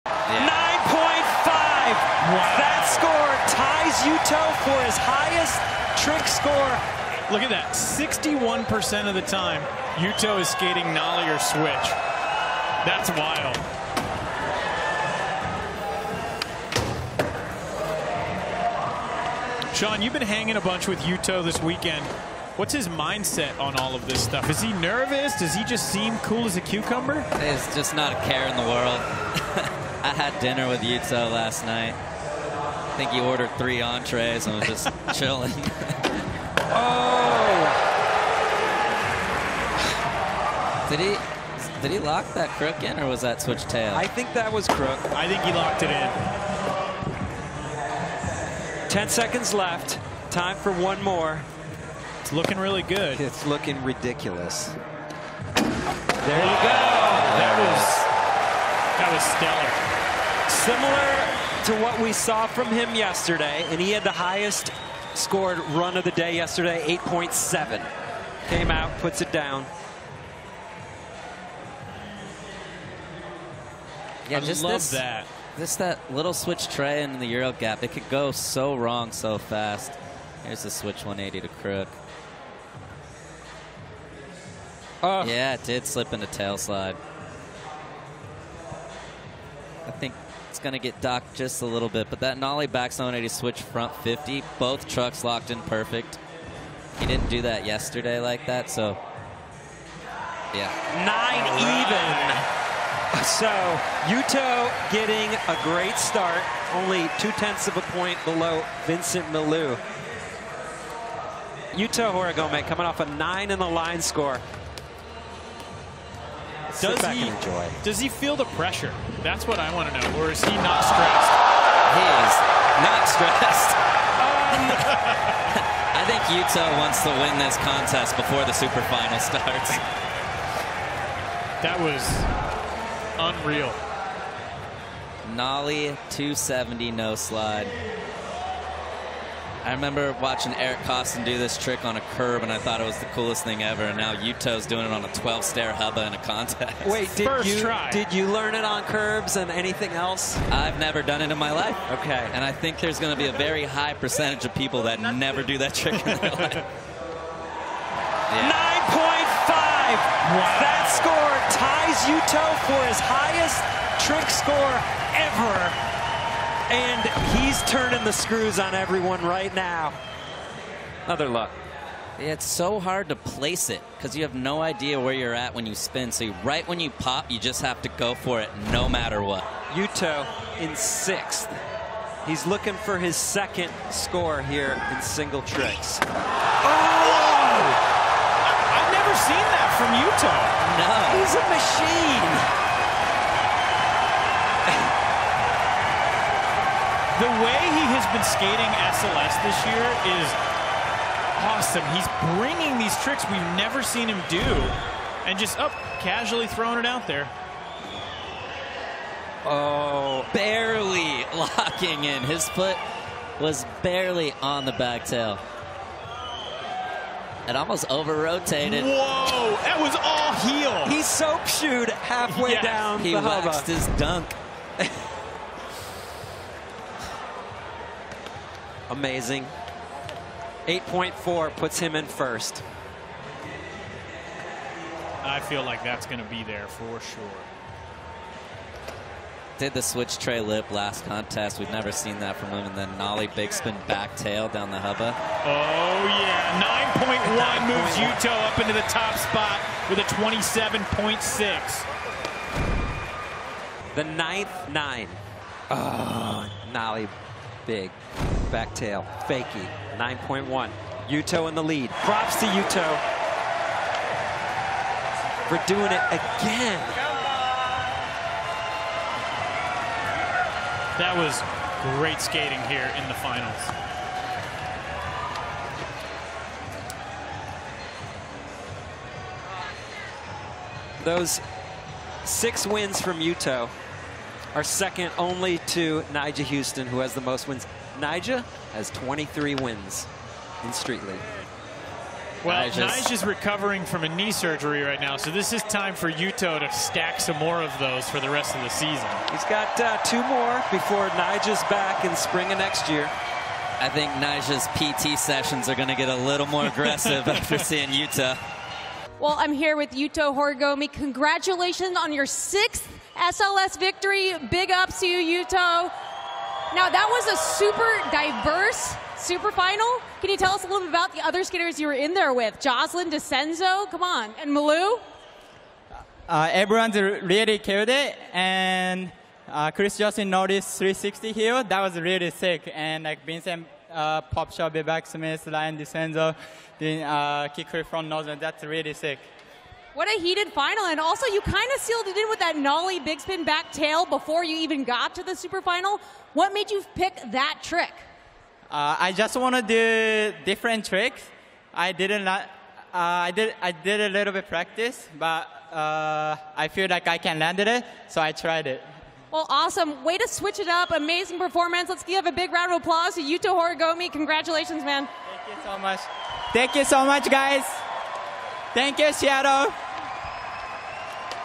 Yuto Horigome SLS Seattle 2022 | Best Tricks
Yuto attacks the Hubba, bringing out a trick that only he can call a "go-to", and breaks out another new one that'll blow your mind.

SLS Seattle is presented by Visible

For the full 2022 SLS Championship Tour schedule, as well as ticket info,